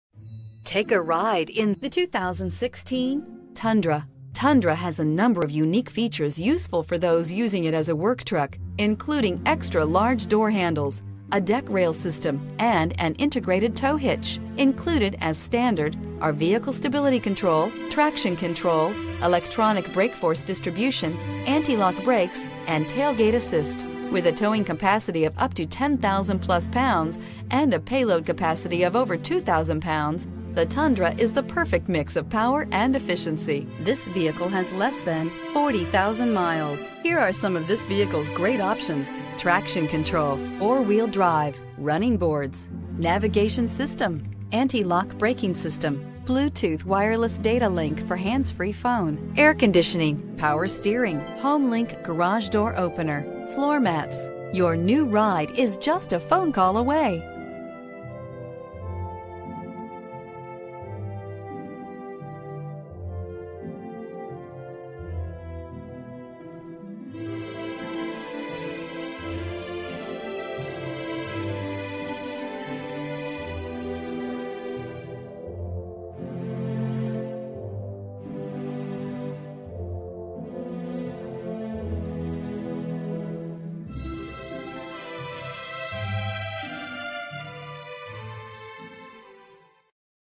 2016 Toyota Tundra 4WD Truck Live Video! Tampa, Wesley Chapel, Brandon, New Port Richey, FL Live P9
2016 Toyota Tundra 4WD Truck LTD

Stadium Toyota
5088 N. Dale Mabry Highway

**Midnight Black Metallic** **Toyota Certifled** **Sunroof** **Heated Leather Seats** **Navigation** Radiating premium good looks and performance to match, our Accident-Free, One-Owner, Certified Pre-Owned 2016 Toyota Tundra Limited CrewMax 4X4 offers an abundance of luxury and capability! Powered by a 5.7 Liter V8 powerhouse provides all the muscle you need, generating 381hp on demand and tethered to a smooth-shifting 6 Speed Automatic transmission. This Four Wheel Drive team provides 18mpg on the open road and acceleration that will get your blood pumping. Prominent alloy wheels, step bars, and a distinct grille accent the strong stance of our Limited, making it impossible to ignore!     The impressive craftsmanship and ergonomic design of the cabin feature heated leather seats, a huge sunroof, premium materials on stylish gauges, and practical buttons and knobs that are refreshingly easy to use. Entune Premium Audio with integrated navigation and app suite keeps you seamlessly connected as you enjoy this comfortable ride. Just try to suppress the smile on your face as this truck works smoothly through its gears propelling you and your family down the road while turning heads.    Our Toyota Tundra leads the pack in safety thanks to intelligently designed ABS, stability/ traction control, trailer sway control, and numerous airbags. For work or play, you deserve an attractive truck that works hard for you.